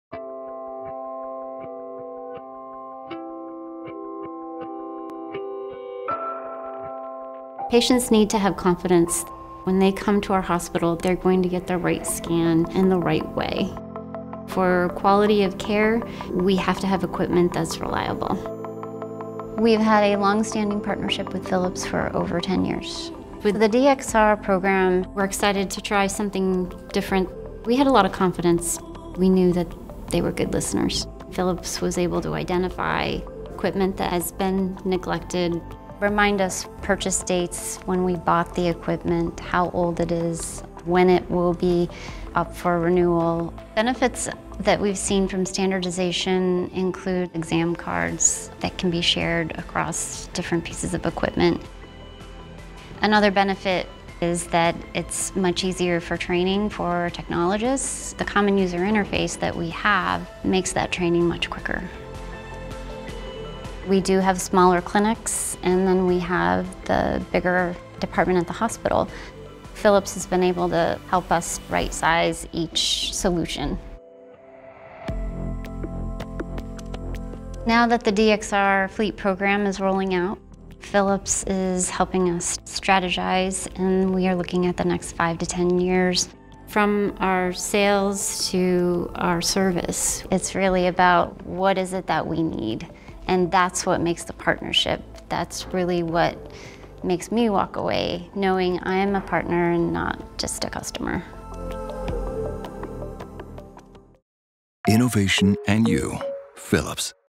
Modernizing a complete X-ray fleet with Philips Diagnostic X-ray technology.
Philips Diagnostic X-ray technology innovation program simplifies the process at University of Vermont Health Network.